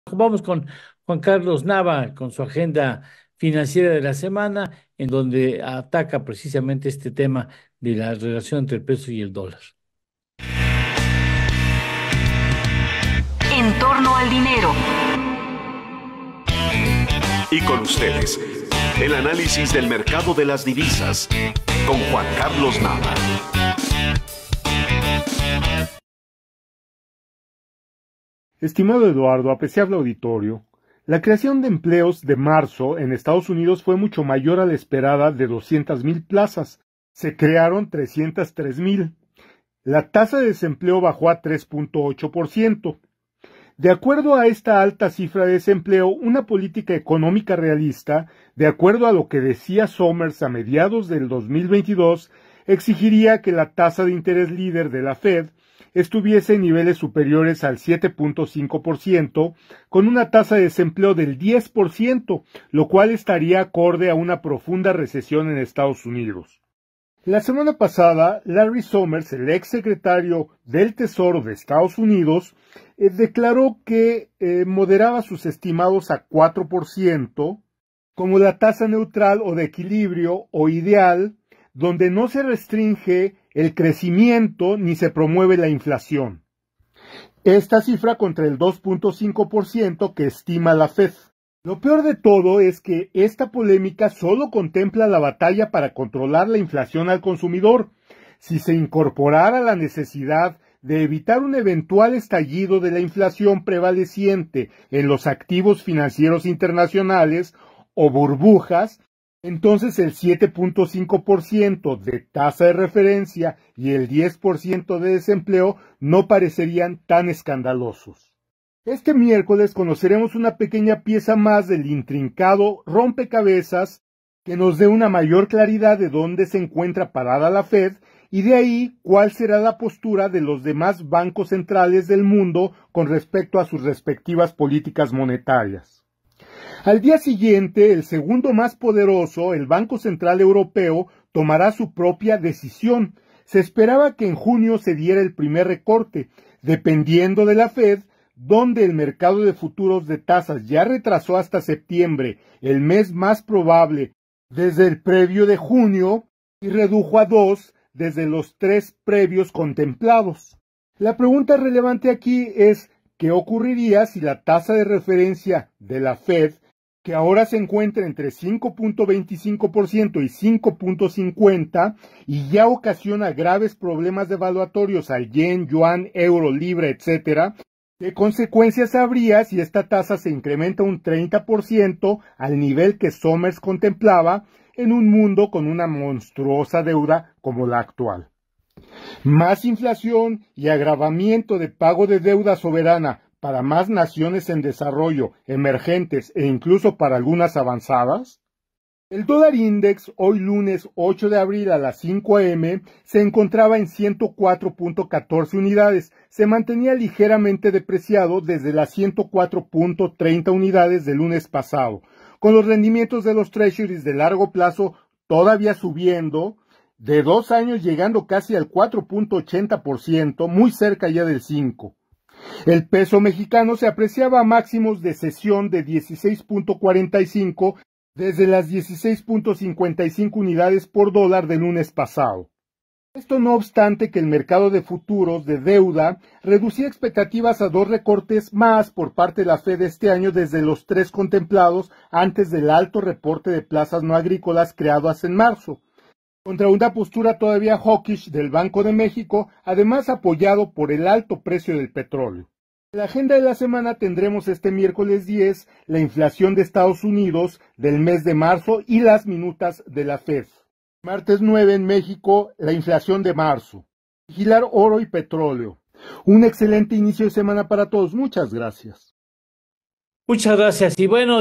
JUAN CARLOS NAVA (8 ABR 24)
EXPERTO EN DIVISAS (Realiza el balance, a principio de semana, en el mercado de divisas, refiere a los temas que en la parte internacional impactan las finanzas nacionales)